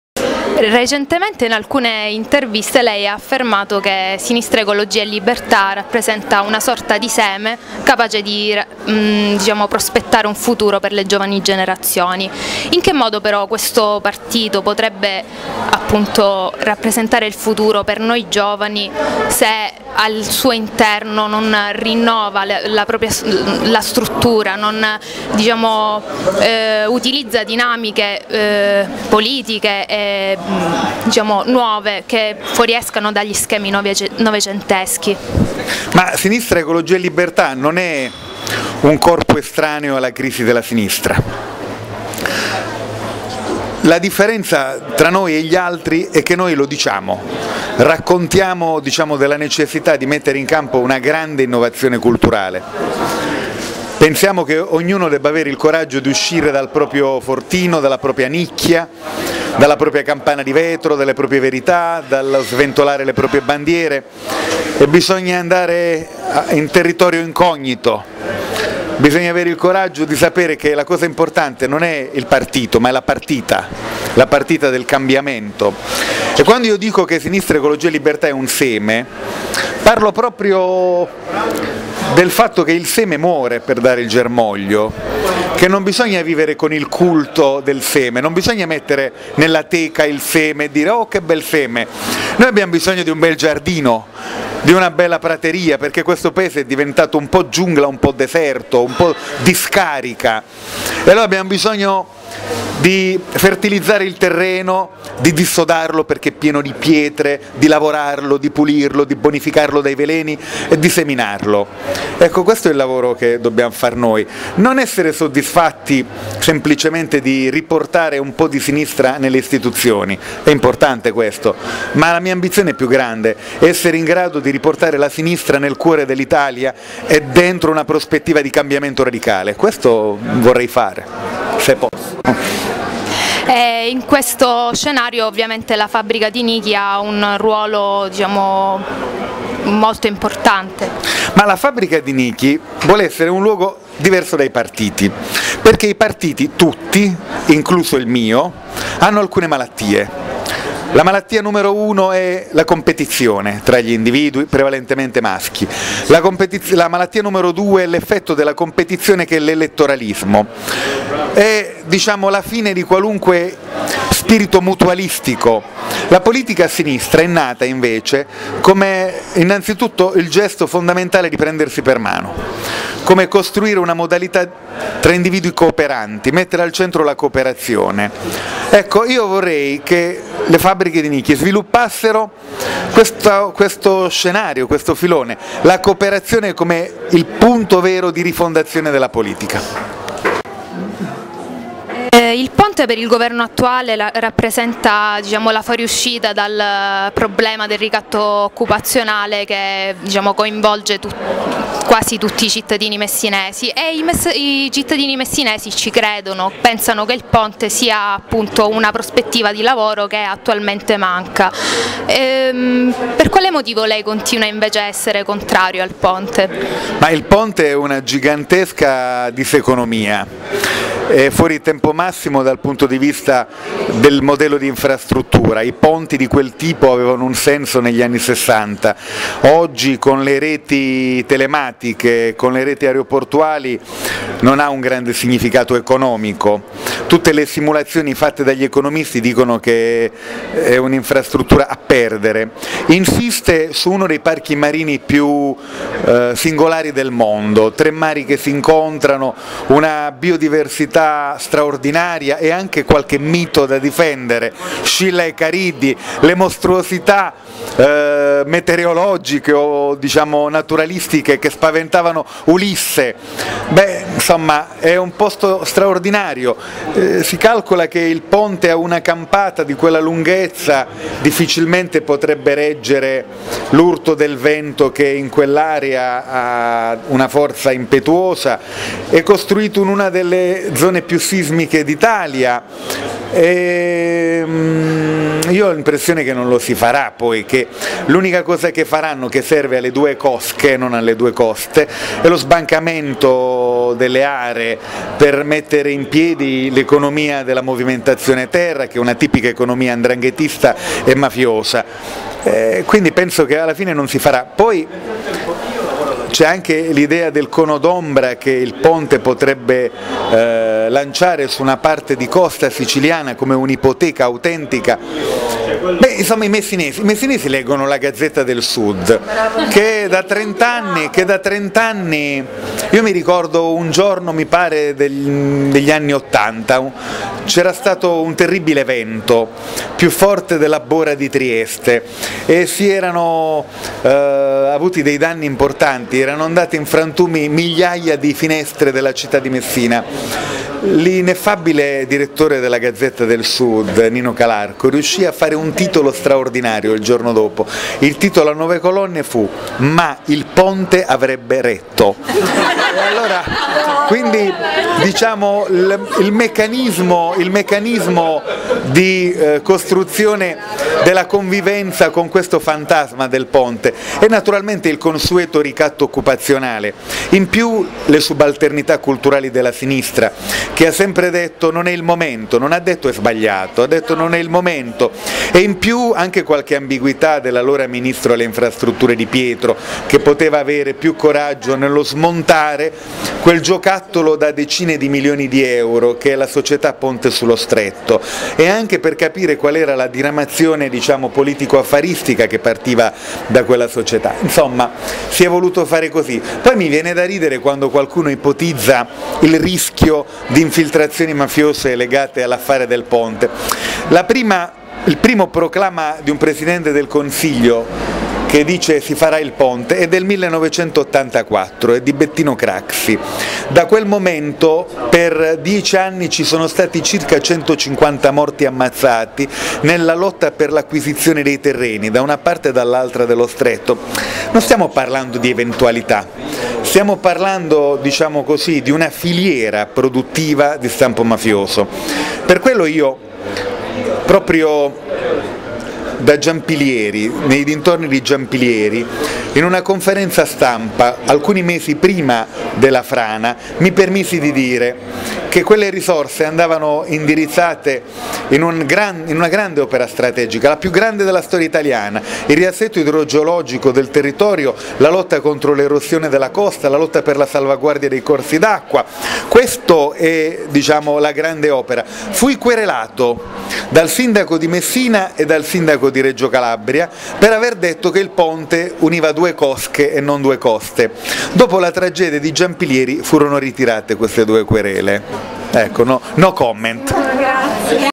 Intervista a Nichi Vendola
Intervista a Nichi Vendola in occasione dell'iniziativa "C'è un sud migliore"-Palermo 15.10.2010